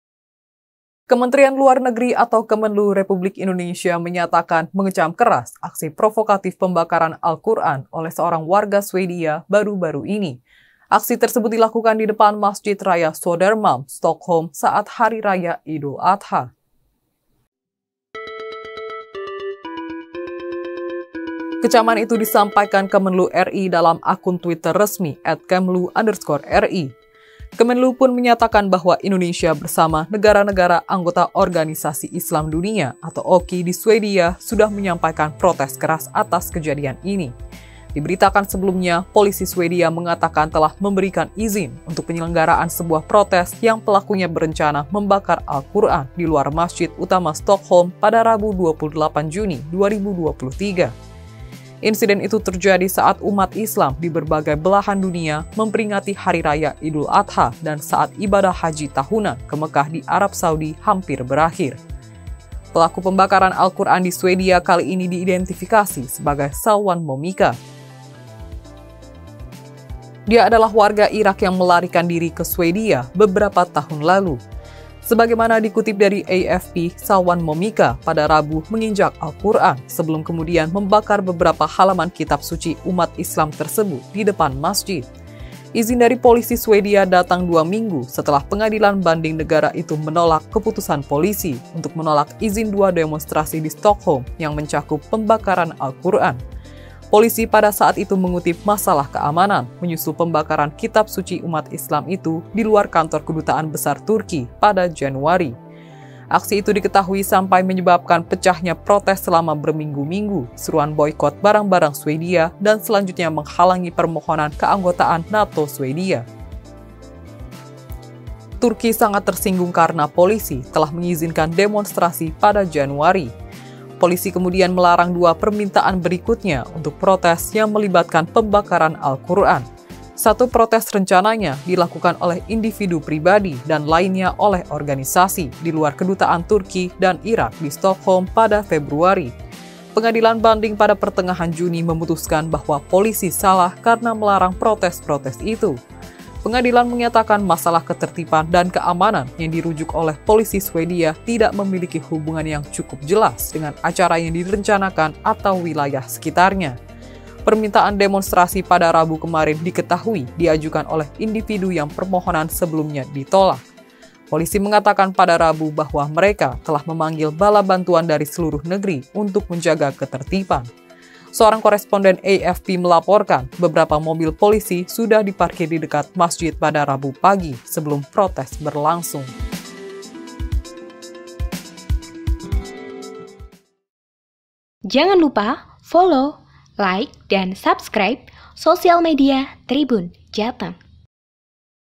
Viral Swedia Izinkan Aksi Bakar Alquran saat Idul Adha
Kementerian Luar Negeri (Kemenlu) Republik Indonesia menyatakan mengecam keras aksi provokatif pembakaran Al Qur'an oleh seorang warga Swedia baru-baru ini.

Aksi tersebut dilakukan di depan Masjid Raya Sodermalm, Stockholm, saat Hari Raya Idul Adha.

Kecaman itu disampaikan Kemenlu RI dalam akun Twitter resmi @Kemlu_RI.

Kemenlu pun menyatakan bahwa Indonesia bersama negara-negara anggota organisasi Islam dunia (OKI) di Swedia sudah menyampaikan protes keras atas kejadian ini.